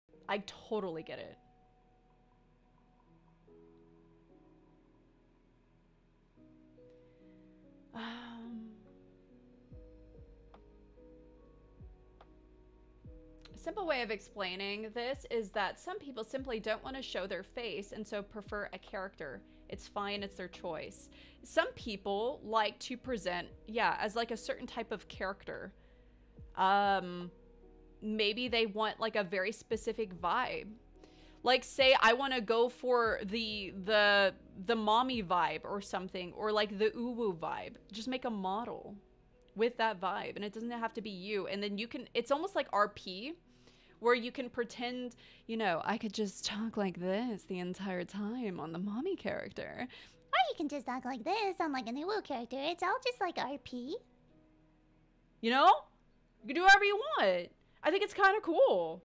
Dizzy kitten talks about what vibe is behind a Vtuber
Full Credit to Dizzy kitten